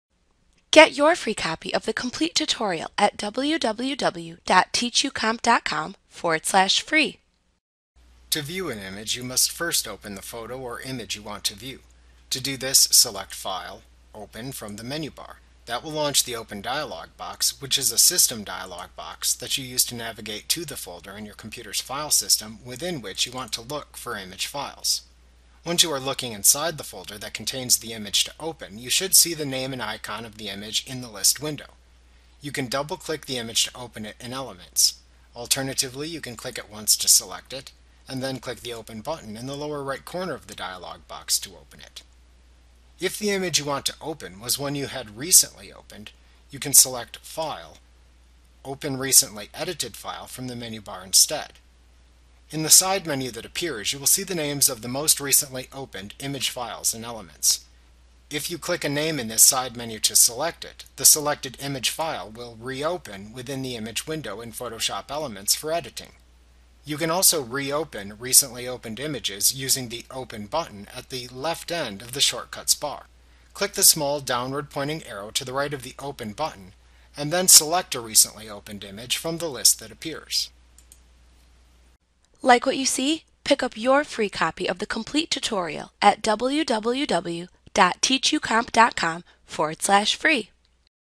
Learn How to Open Images in Adobe Photoshop Elements 2023: A Training Tutorial
FREE Course! Learn how to open files in the Photo Editor in Photoshop Elements 2023 A clip from Mastering Photoshop Elements Made Easy. - the most comprehensive Photoshop Elements tutorial available. Visit us today!